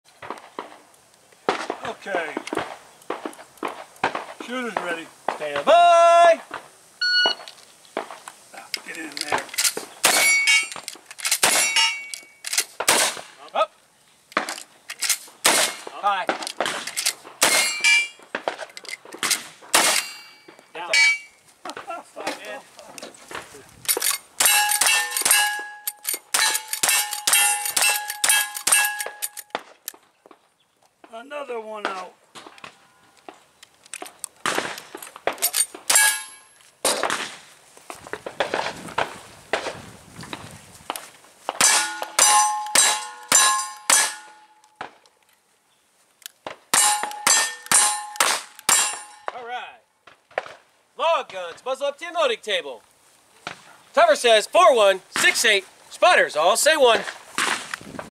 Stage 2 Polecat Outlaws 4 24 21
OK Corral Outlaws April Monthly Match at the OK Corral Gun Club in Okeechobee, FL.

Top 5 Overall:
1) The Outlaw Travis James - WRA - 120.39
2) Crane Creek Kid - COWB - 127.65
3) Angus McNasty - 49er - 153.81
4) Thunderbolt Hayes - S - 170.57
5) Jeremiah Longknife - ES - 176.44

Top 3 Ladies:
1) Chickie Winsome
2) Velvet Sheath
3) Wild Ruth McCoy

Clean Shooters:
Chickie Winsome and The Outlaw Travis James

Dagnabbit:
Badlands Barry and Crane Creek Kid.